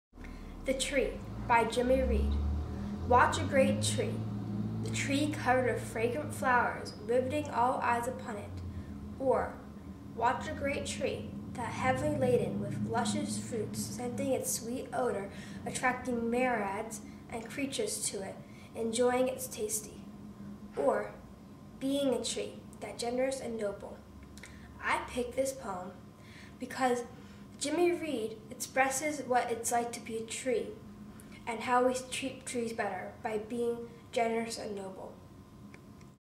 The Tree by Grace A
Grace A recites The Tree by Jimmy Reed